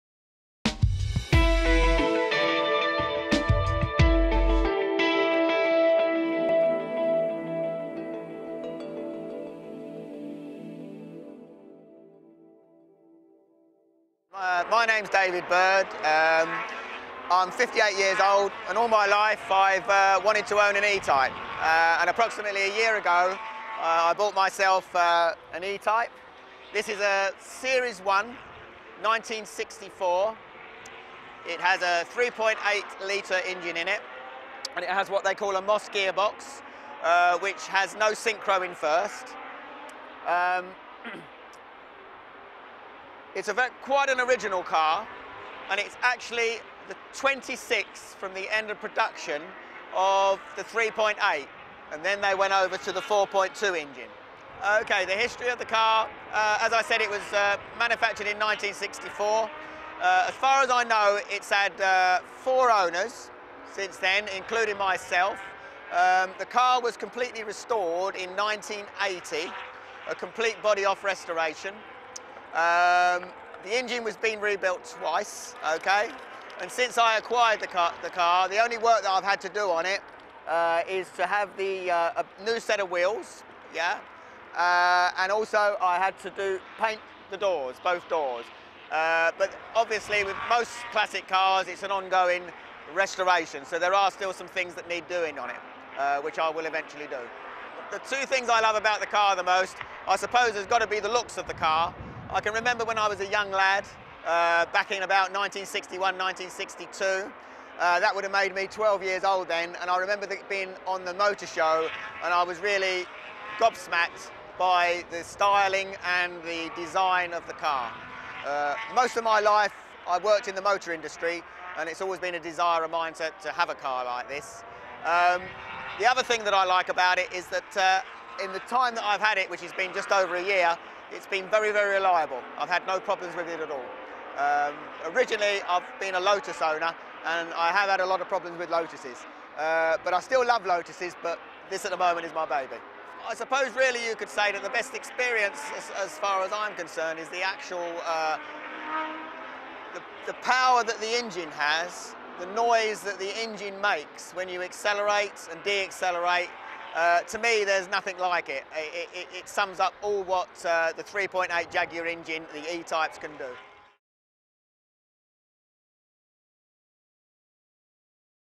Jaguar E-Type 1964 - Silverstone Classic. CarshowClassic.com
Hear David tell the story of his 1964 Jaguar E-Type. Enjoy more iconic classic supercars from Ferrari, Porsche, Aston Martin, Mercedes, Maserati, Jaguar, Lotus, Triumph, MG and many more